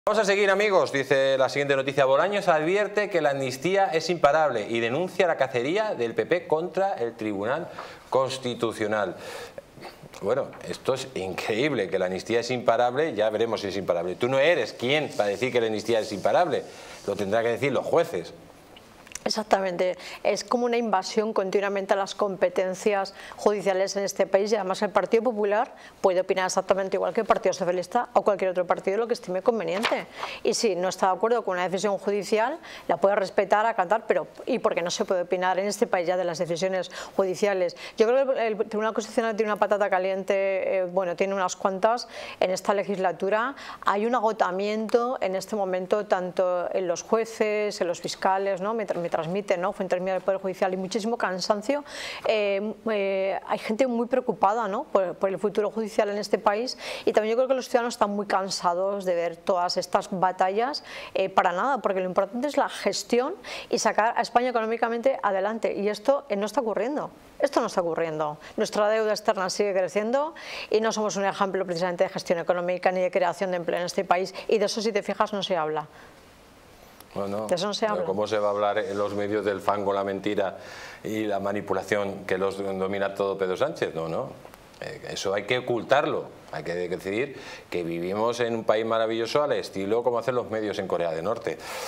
🔴Bolaños advierte que la amnistía es “imparable” y denuncia la “cacería” del PP contra el TC🔴
¡No te pierdas la última noticia sobre la amnistía en España! Bolaños advierte que es "imparable" y denuncia la "cacería" del PP contra el TC. ¡Mantente informado con Distrito TV! 🔴
🔴Conviértete en miembro de este canal para disfrutar de ventajas: